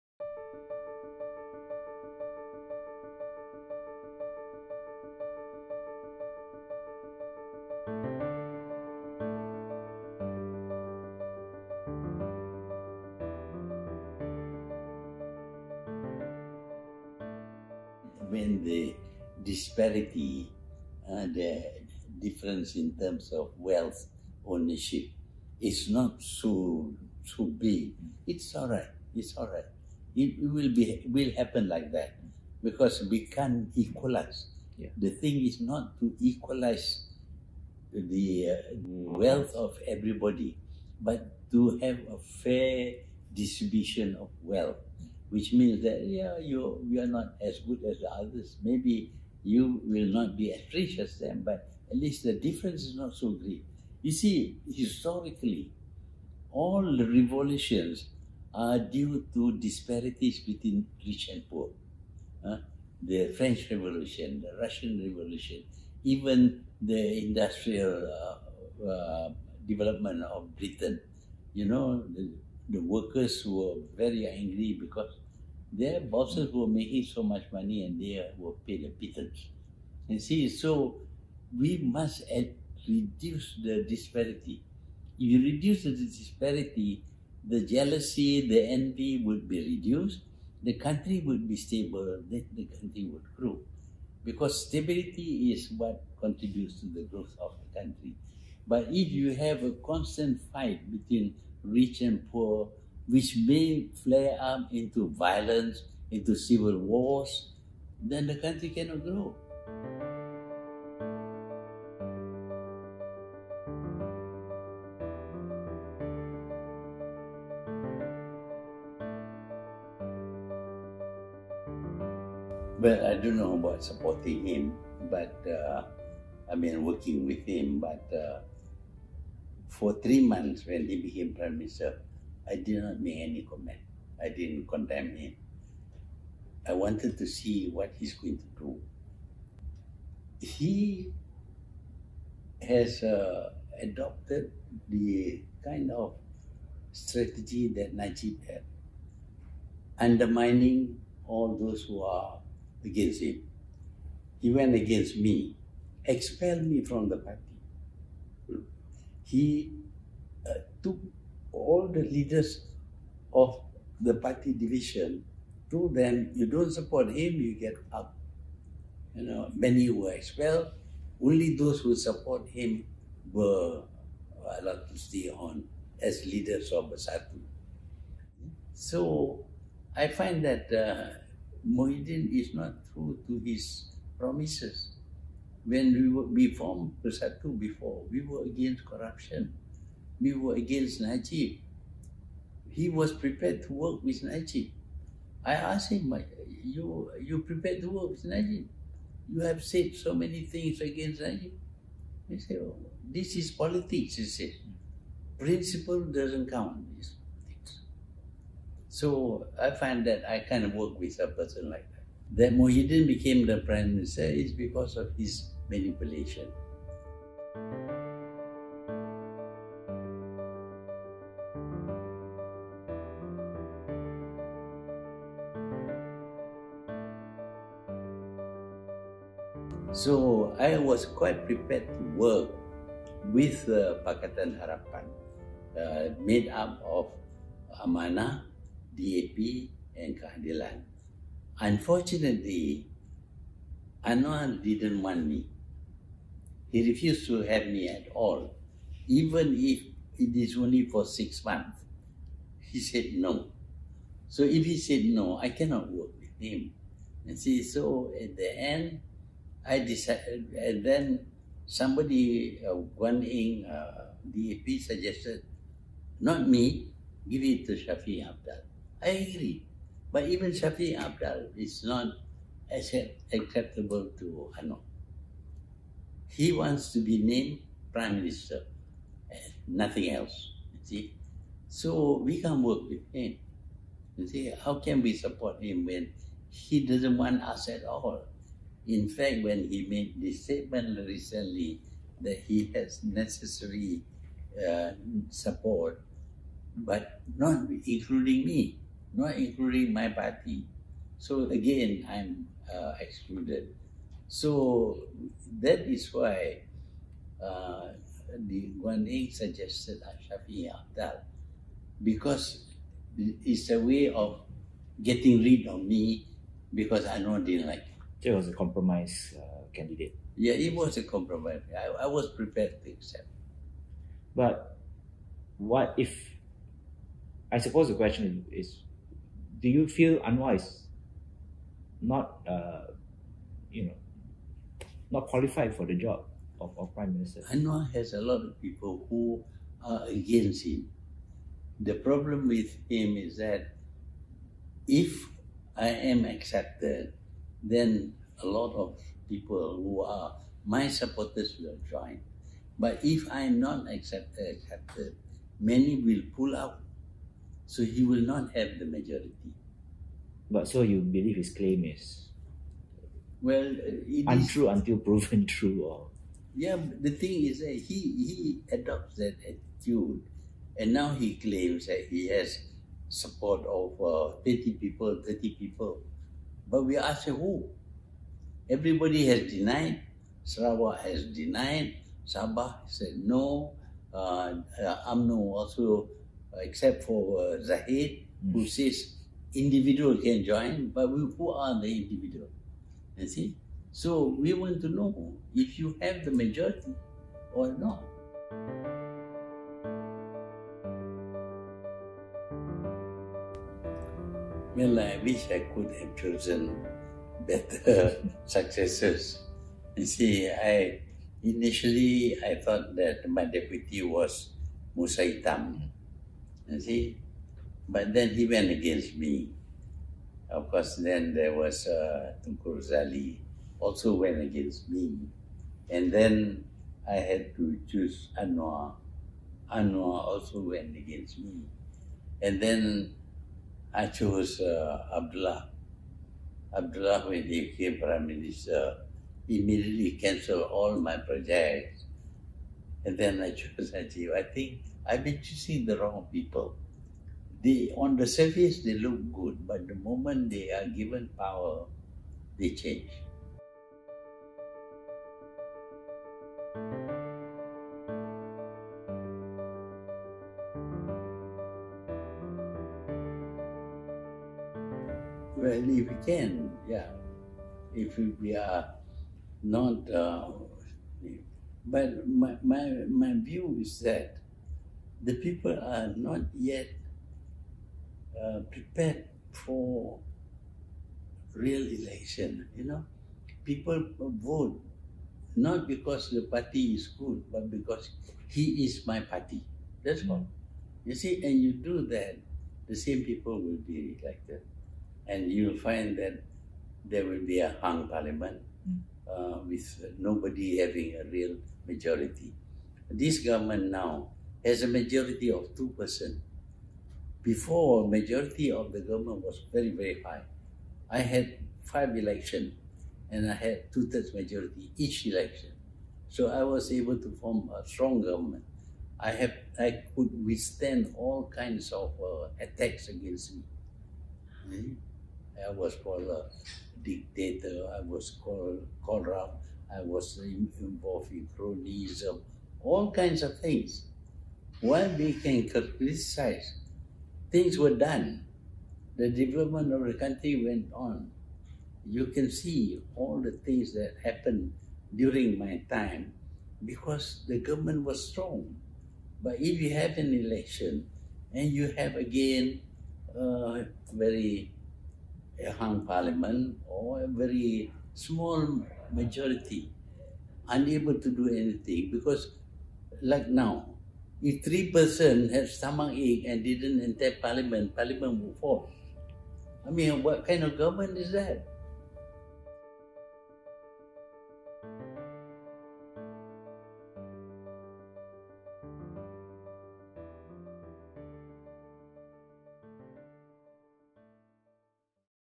The ST interview: Mahathir on Muhyiddin, regrets and future elections
Former Malaysian premier Mahathir Mohamad could yet lead his two-month-old party, as well as the opposition, into the next general election, as he revealed that supporters still want him to continue as an elected representative. ST Malaysia correspondent Shannon Teoh interviews former Malaysia prime minister Mahathir Mohamad. Read the story